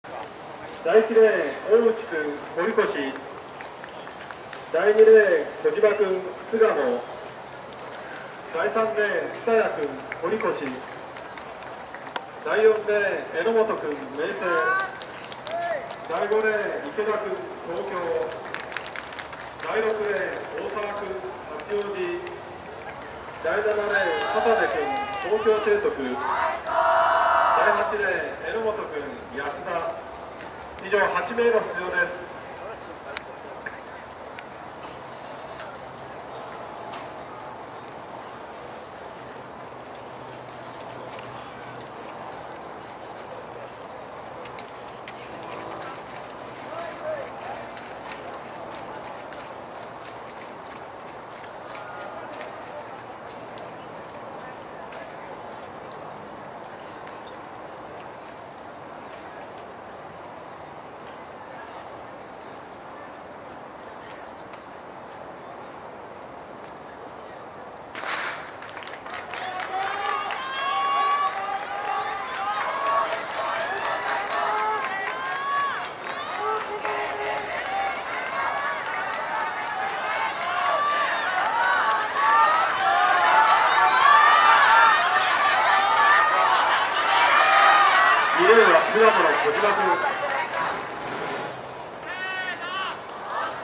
20120930 都新人男子200m決勝
2012年9月30日第64回東京都高等学校新人陸上競技対校選手権大会3日目@大井ふ頭中央海浜公園陸上競技場

1位　(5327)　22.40小嶋　健太郎(2)巣鴨 ・３支部
2位　(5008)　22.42榎本　大樹(2)明星 ・５支部
3位　(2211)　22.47大澤　裕輝(2)八王子 ・６支部
4位　(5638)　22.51榎本　圭祐(2)安田 ・２支部
5位　(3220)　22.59池田　亮平(2)東京 ・１支部
6位　(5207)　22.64大内　裕太郎(2)堀越 ・４支部
7位　(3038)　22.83笹部　海人(2)東京成徳 ・３支部
8位　(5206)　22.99蔦谷　隆也(2)堀越 ・４支部